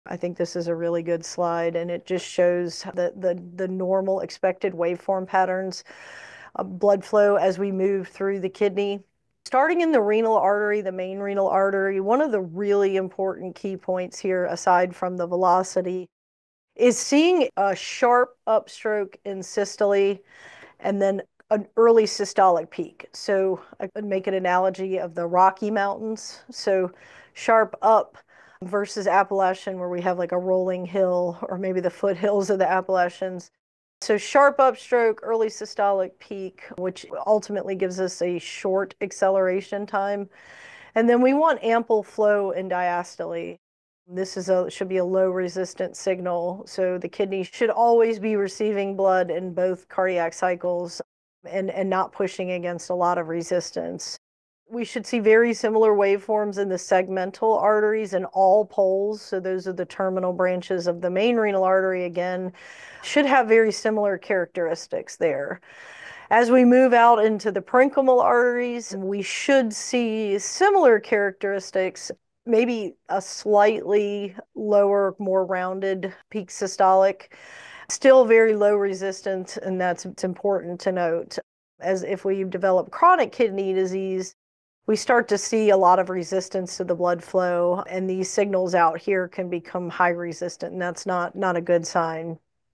Renal Artery Stenosis-Sample of Training Video
🔍 Weekly Ultrasound Training Spotlight from GCUS!
🎥 Presented by: Shannon York, BS, RVT, RDCS
This week, expand your vascular imaging expertise with "Ultrasound Evaluation of Renal Artery Stenosis" — a focused training video offering a comprehensive overview of duplex/color flow techniques using both direct and indirect evaluation methods.
✨ What You'll Learn:
 ✅ Understand renal artery stenosis and the role of ultrasound in diagnosis
 ✅ Review anatomy, scanning techniques, diagnostic criteria, and incidental findings
 ✅ Learn to perform and interpret calculations for both direct and indirect methods
Ideal for physicians, sonographers, PAs, NPs, and other professionals in radiology, vascular surgery, cardiology, internal medicine, and primary care, this training equips you with practical tools for confident renal artery assessment.
🎓 Bonus: Enjoy lifetime access to this training video so you can review it anytime, at your own pace!